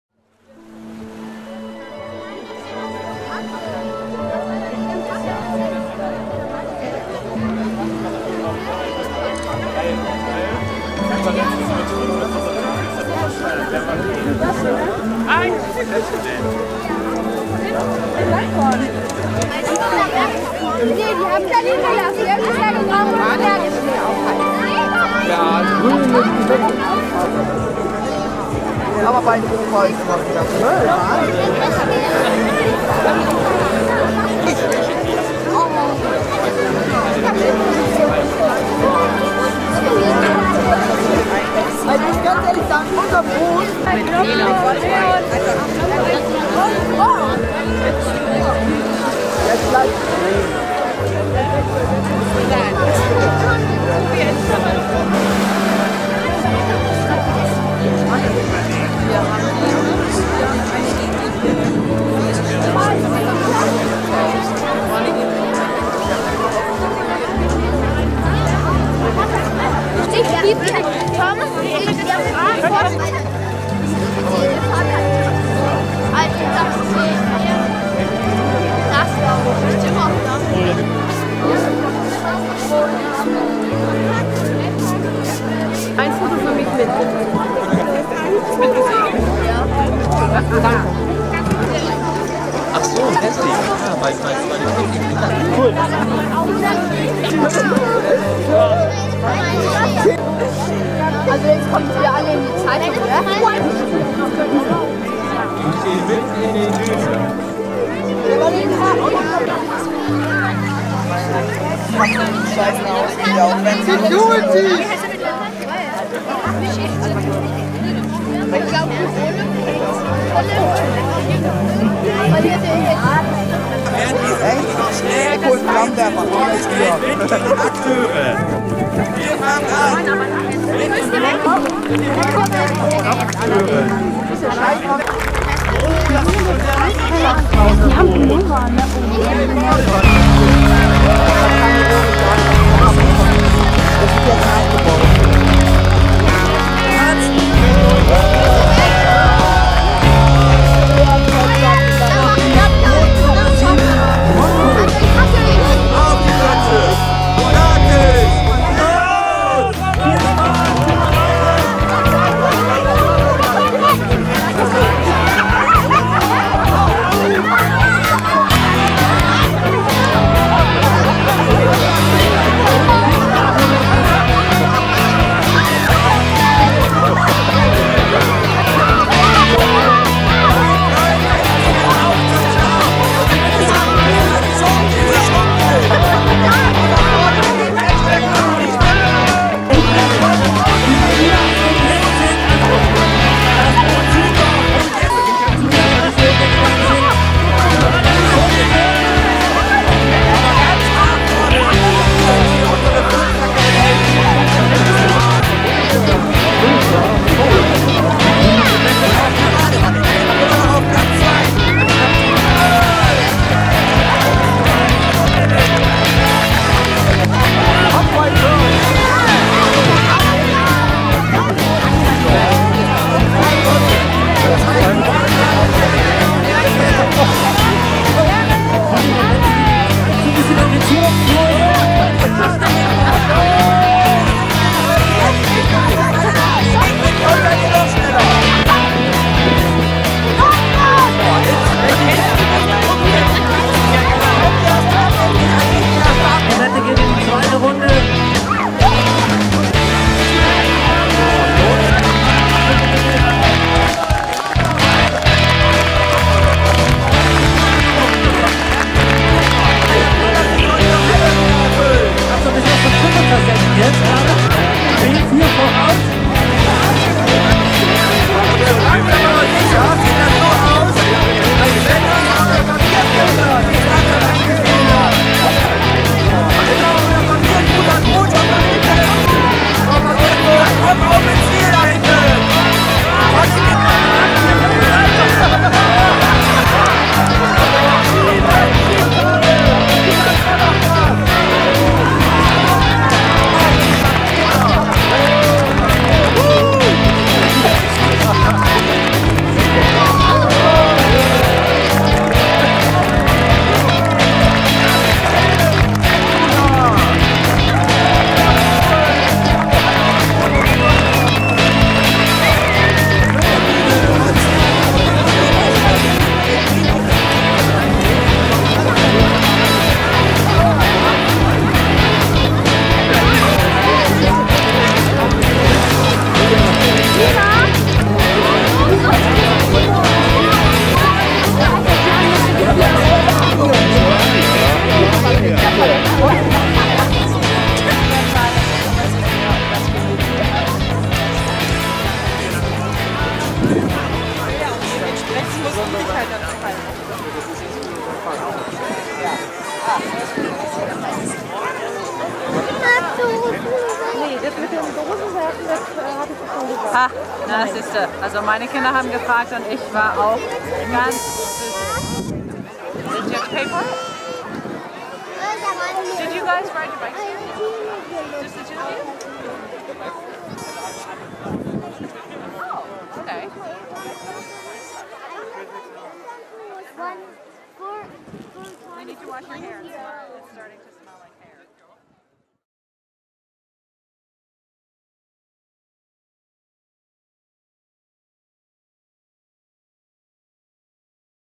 Papierbootrennen Der Film Amrum 2014
Spannendes Papierbootrennen in der Süddörfer Mole! Hohe Kreativität und Ingenieurskunst wird verlangt. Ein wunderbares Spektakel für Jung und Alt. Wie lange hält sich das beste Papierboot über Wasser? Topevent auf der Nordseeinsel Amrum im August 2014